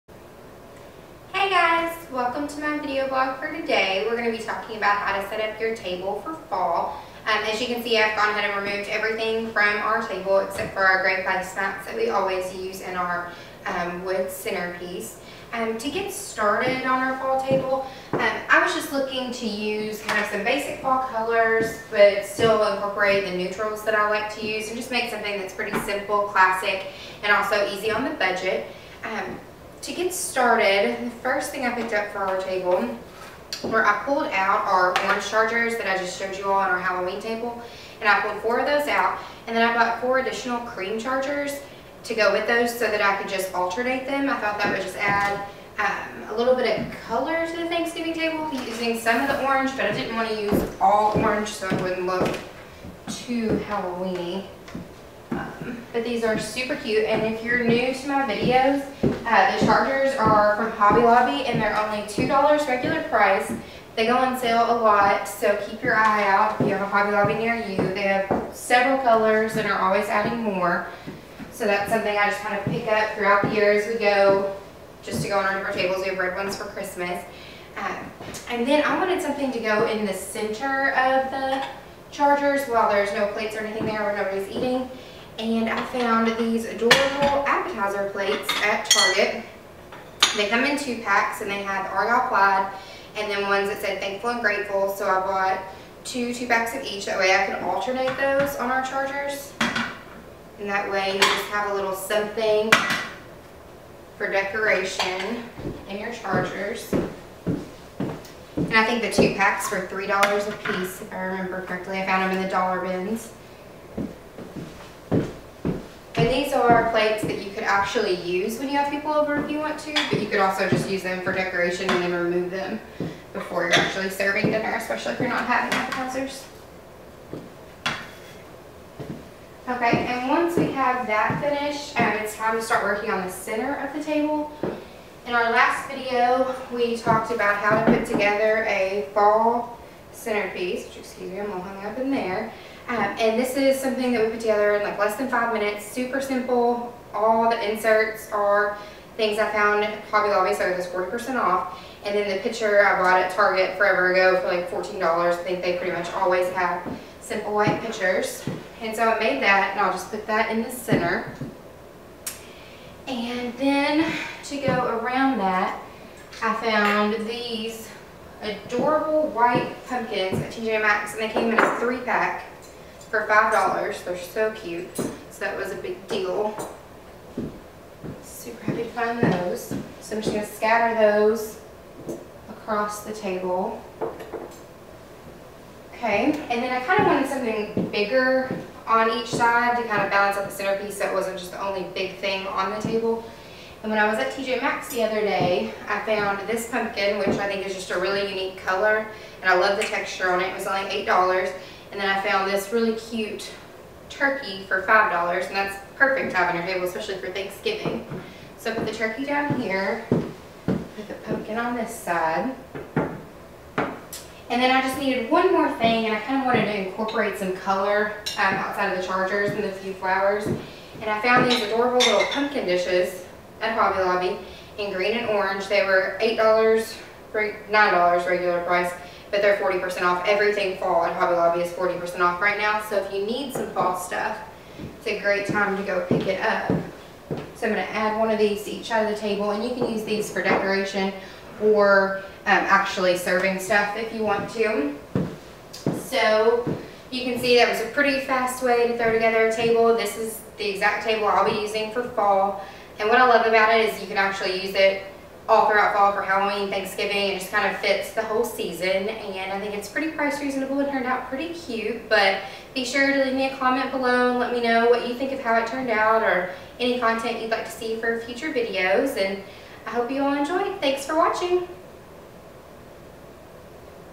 How to Decorate your Fall Table / The Peacock Roost
How to decorate your table for fall!  Using items from Target, Hobby Lobby & TJ Maxx decorate your table in a cute and easy way that will last the whole fall season!

❤ Follow Along! ❤